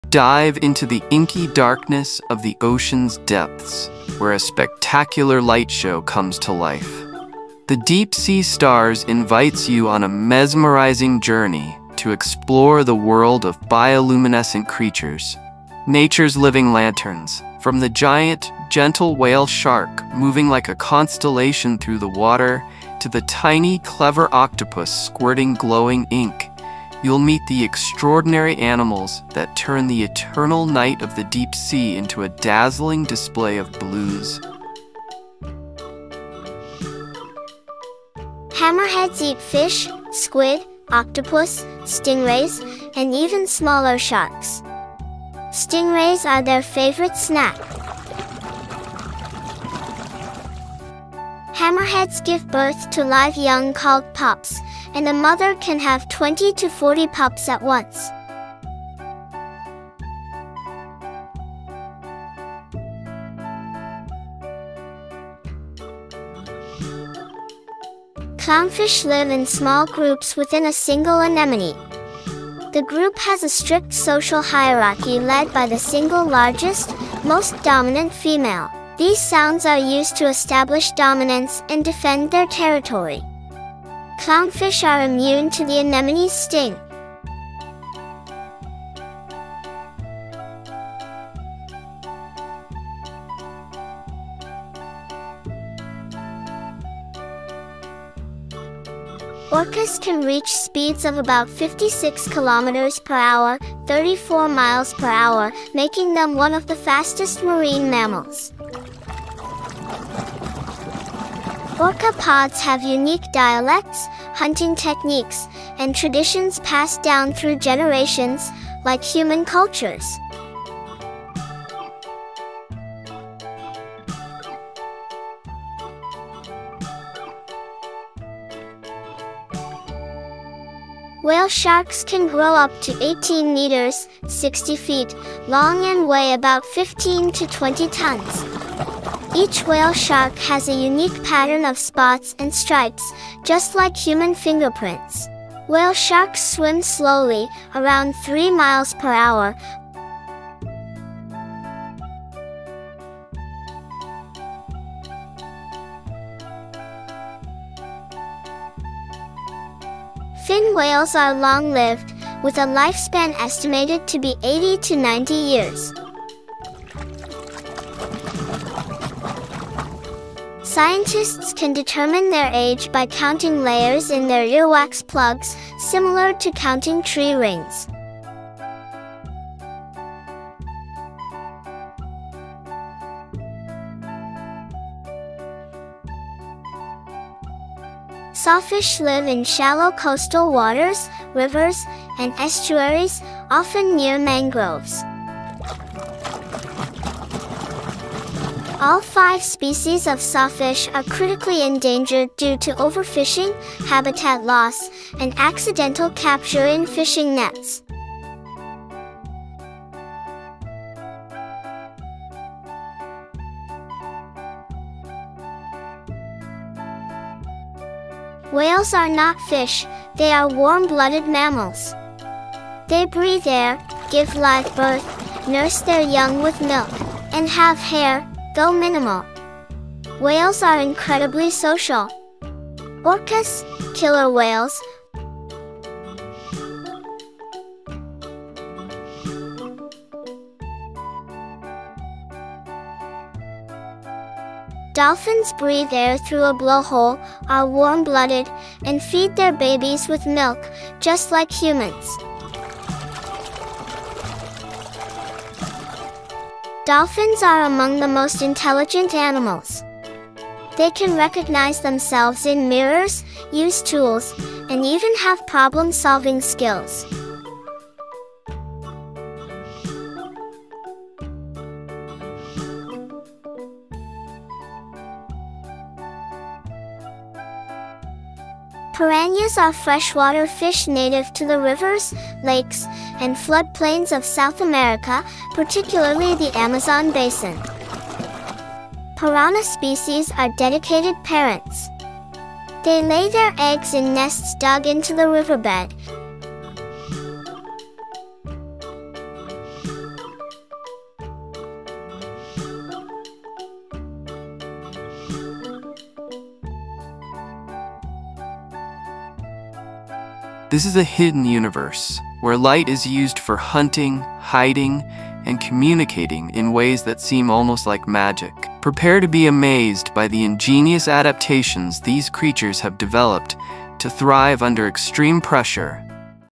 The Deep Sea Stars: A Sparkling Journey into the Glowing Sea Creatures🐋🦀
Dive into the inky darkness of the ocean's depths, where a spectacular light show comes to life! The Deep Sea Stars invites you on a mesmerizing journey to explore the world of bioluminescent creatures—nature's living lanterns. From the giant, gentle whale shark moving like a constellation through the water to the tiny, clever octopus squirting glowing ink, you'll meet the extraordinary animals that turn the eternal night of the deep sea into a dazzling display of blues, greens, and reds. This is a hidden universe, where light is used for hunting, hiding, and communicating in ways that seem almost like magic.

Prepare to be amazed by the ingenious adaptations these creatures have developed to thrive under extreme pressure and without sunlight. You'll learn how the anglerfish uses a glowing lure to attract unsuspecting prey, how the vampire squid creates a dazzling distraction to confuse predators, and how countless tiny organisms light up the waves with every splash, creating shimmering seas. Interactive exhibits allow you to experiment with how light travels underwater, create your own bioluminescent signals, and even navigate a dark maze using only "glowing" clues, just like the creatures of the deep.

More than just a tour of nature's wonders, this journey highlights the fragility of these deep-sea ecosystems and the importance of protecting them from threats like pollution and deep-sea mining. The Deep Sea Stars aims to ignite a sense of wonder and responsibility, showing how these incredible, glowing beings are not just scientific curiosities but vital parts of our planet's biodiversity. You'll leave with a newfound appreciation for the ocean's mysteries and the sparkling stars that call the abyss their home.

Enjoy more enjoyable videos!